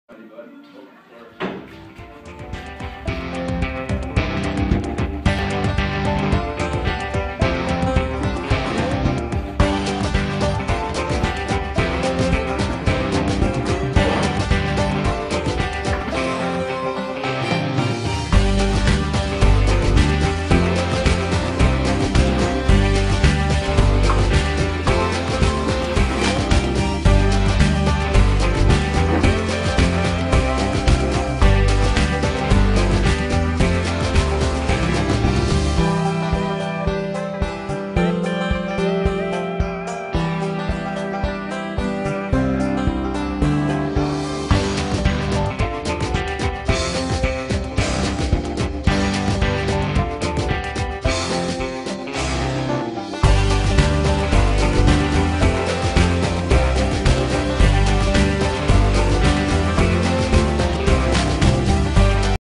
Counter Measures: Installing Granite at 300 Broadway, Downtown Fargo
Kilbourne Group, a downtown revitalization company, believes anything worth doing is worth doing well. Likewise, our latest condos at 300 Broadway in Downtown Fargo include high-end finishes -- like the gorgeous, black-and-gold granite countertops being installed here. Just 4 units remain in this exceptional building; this one will be done next week!